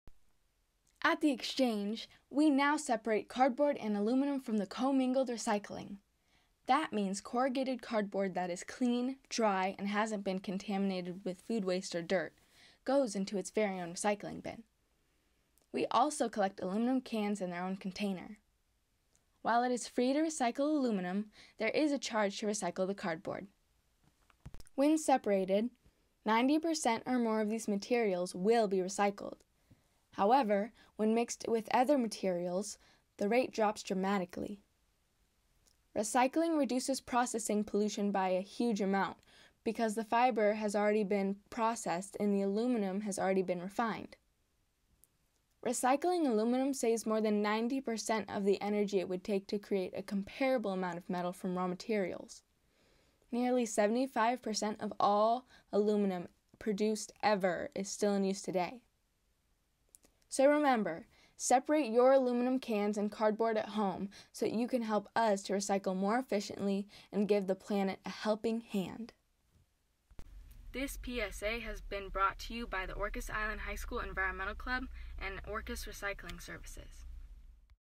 Cardboard and Aluminum Recycling
PSA explaining how to recycle cardboard and aluminum at ORS/The Exchange on Orcas Island.